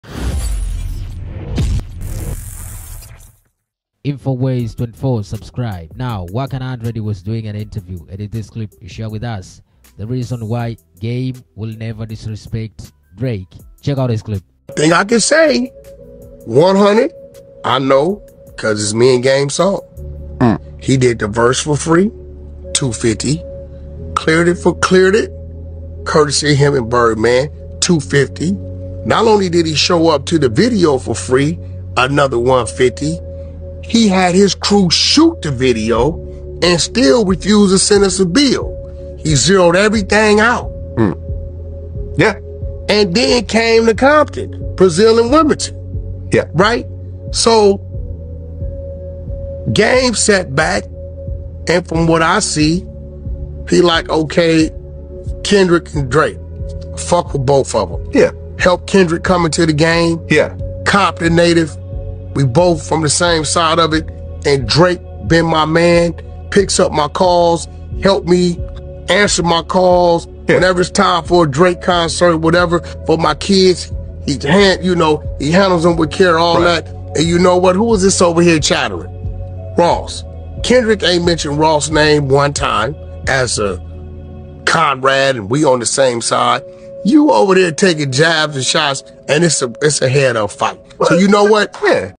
Wack100 REVEALS Why The Game is Over LOYAL to Drake.
Wack100 talks on The Game Loyalty to Drake.